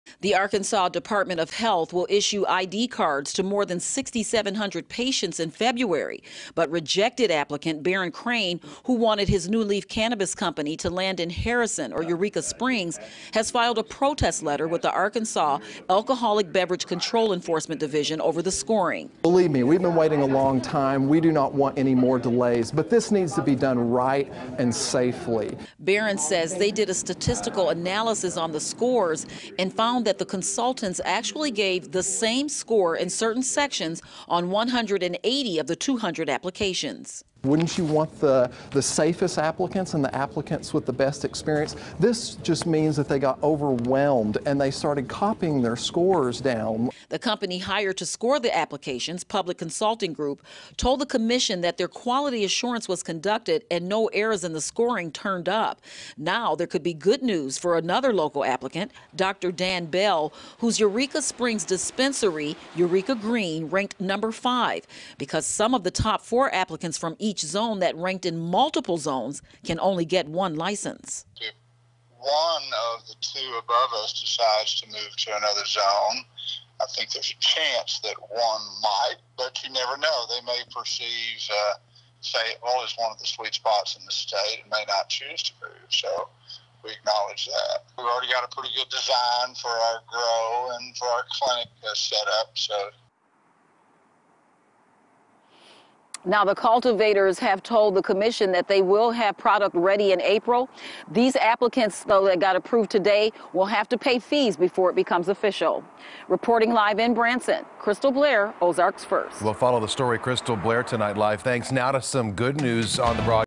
Dispensaries for Medical Marijuana Approved in Arkansas
ARKANSAS -- In a unanimous vote, the Medical Marijuana Commission approved the scores for 200 dispensaries in Arkansas, Wednesday evening.

The Arkansas Department of Health will issue identification cards to more than 6700 patients in February. Rejected applicant, Baraon Crane, who wanted his "New Leaf Cannabis Company to land in Harrison or Eureka Springs, has filed a protest letter with The Arkansas Alcoholic Beverage Control Enforcement Division, over what he says is a scoring issue.

"Believe me, we've been waiting a long time," said Crane, "We don't want any more delays, but this needs to be done right and safely."

Crane says they did a statistical analysis on the scores and found that the consultants actually gave the same score in certain sections on 180 of the 200 applications.

"Wouldn't you want the safest applicants and the applicants with the best experience? This just means that they got overwhelmed and started copying their scores down," Crane said.

The company hired to score the applications, Public Consulting Group, told the commission that their quality assurance was conducted and no errors in the scoring turned up.

There could be some good news for another local applicant. Dr. Dan  Bell, whose Eureka  Springs dispensary ranked number five. Some of the top four applicants from each zone, that ranked in multiple zones, can only get one license.

"If one of the two above us, decides to move to another zone,  I think there's a chance that one might. But you never know," said Dr. Bell, "They may perceive Fayetteville as one of the sweet spots in the state and may not choose to move. So, we acknowledge that."

"We've already got a good design for our grow and for our clinic that's set up..so."

Cultivators told the commission that they should have the product ready in April.

Applicants will have to pay fees before they can officially start business.